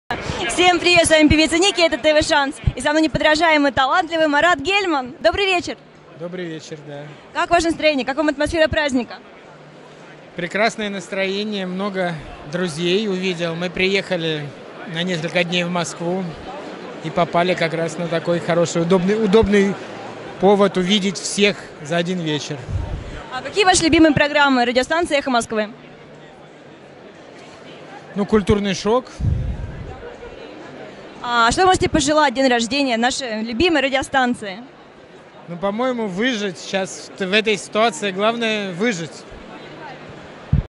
Интервью Марата Гельмана - российский коллекционер, галерист и публицист, арт-менеджер
1 ноября 2018 года радиостанция «Эхо Москвы» отметила свой день рождения.  28 лет в эфире!  В Галерее искусств Зураба Церетели где традиционно проходит праздник, съехались именитые персоны.